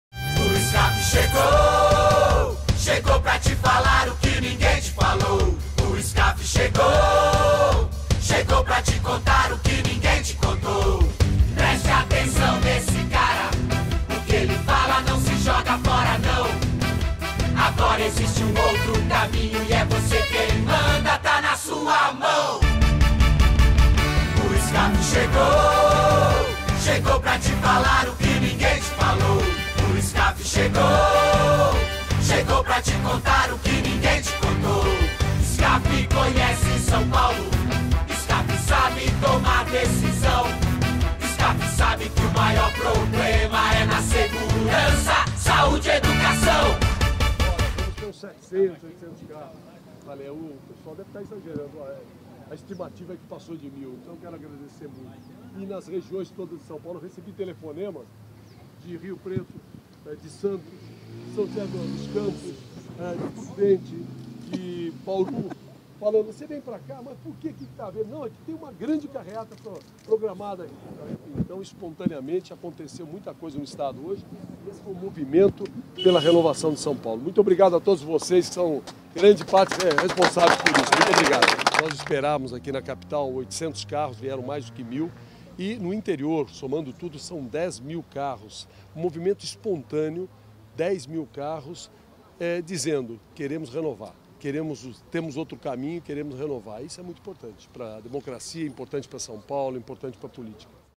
Carreata do Skaf reúne dez mil carros no Estado
Carreata do Skaf percorre São Paulo: Milhares de pessoas reuniram-se em 70 cidades do estado todo neste sábado (13/09) para levantar a bandeira de renovação